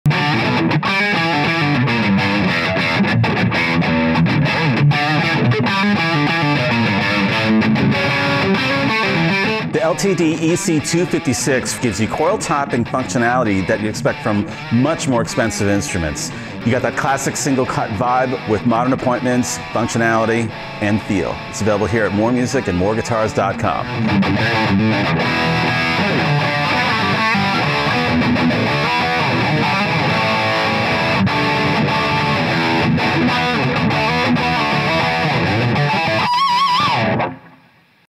ESP LTD EC-256 | Vintage Natural
Guitars in the LTD 200 Series offer excellent value and provide higher quality than nearly any other instruments in their price range. The EC-256 Vintage Natural is a set-neck design with a mahogany body and great-looking flamed maple top, along with a comfortable and easy-to-play thin U-shaped three-piece mahogany neck and roasted jatoba fingerboard. It features a set of acclaimed ESP Designed LH-150 humbucker pickups with great tone for all kinds of music from rock to pop to metal, and for even more sonic versatility, the pickups can be split into single-coil with a push-pull control.